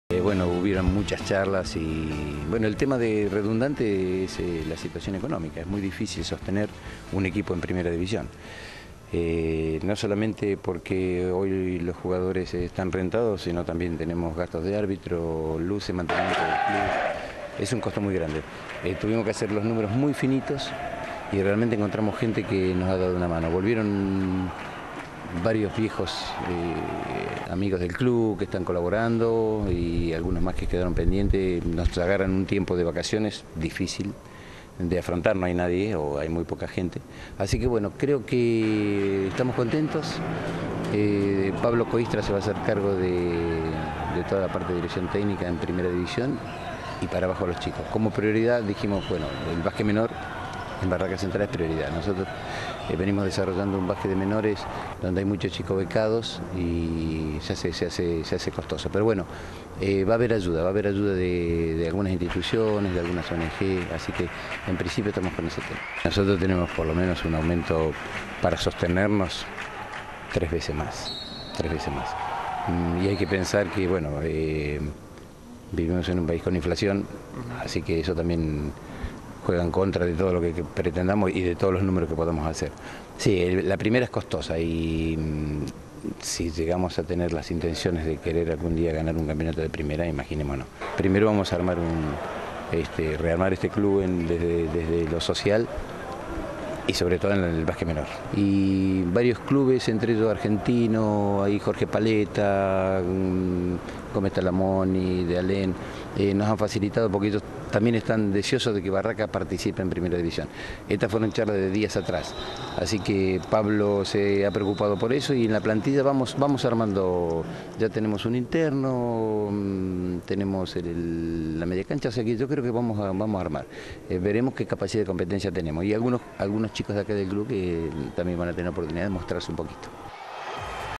Canal 9 Bahía Blanca - NO SE BAJA
MÁS ALLÁ DE LAS VERSIONES QUE SURGIERON A PRINCIPIOS DE AÑO, BARRACAS DECIDIÓ PRESENTAR EQUIPO Y JUGARÁ EL OFICIAL DE PRIMERA DE BASQUET.